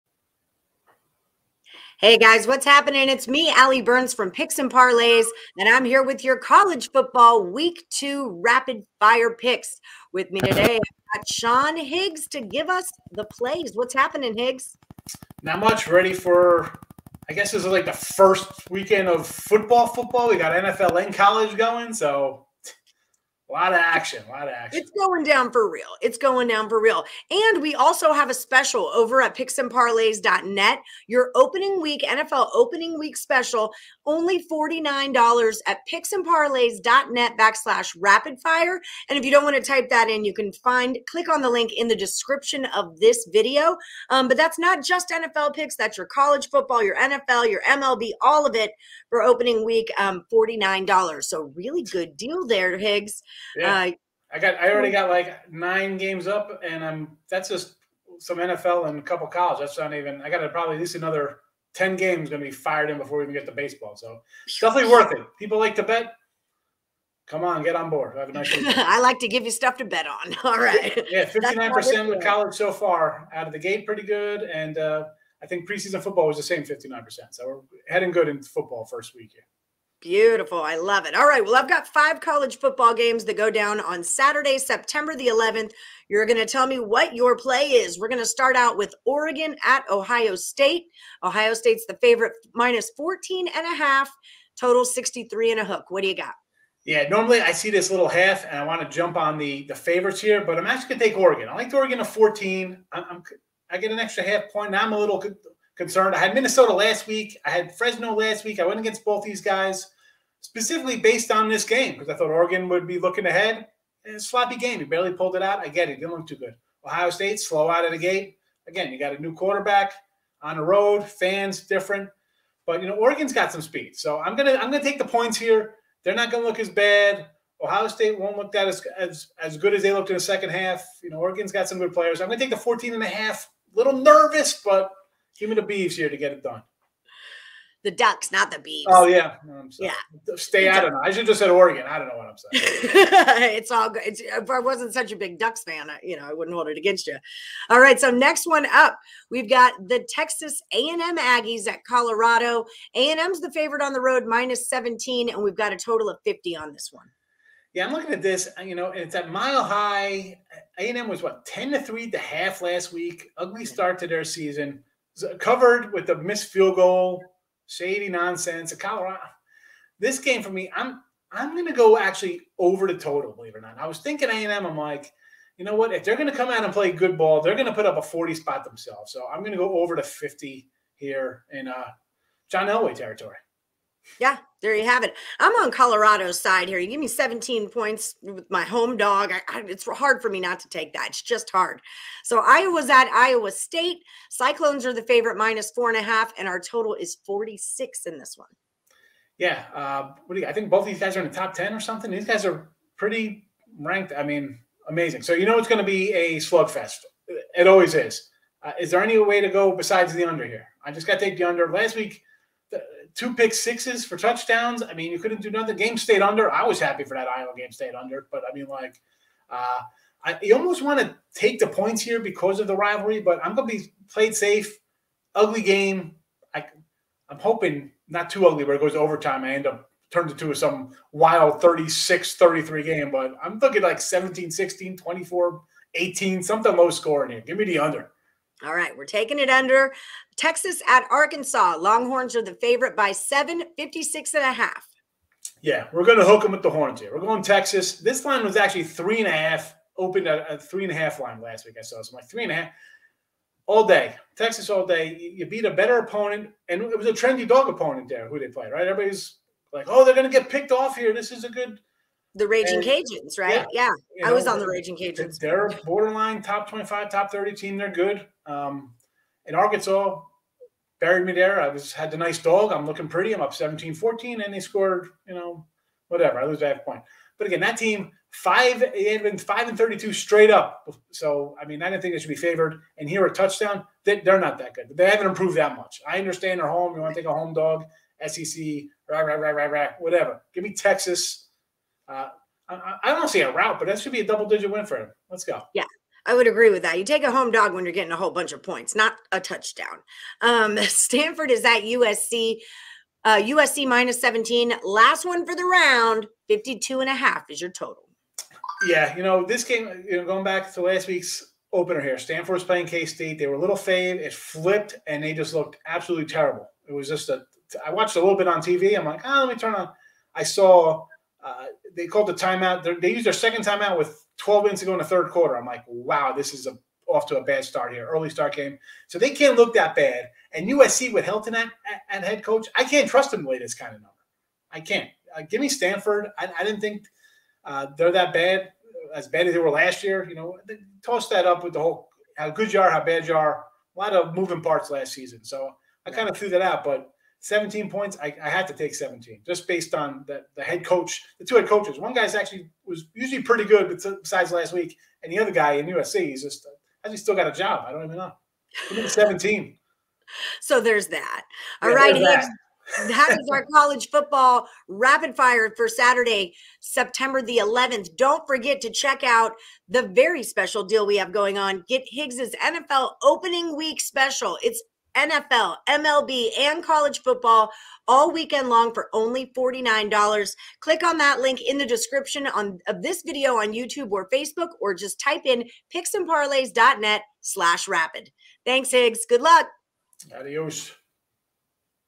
NCAA Football Rapid Fire - Week 2 Betting Picks & Predictions | Picks & Parlays
Get Sean's Special NFL kickoff sale - All Picks through Monday - Just $49

Football is Here And We have got Your Betting Needs in this Week 1 Rapid Fire for Saturday September 11th, 2021 with Picks and Parlays Expert Handicappers Ali Burns and Sean Higgs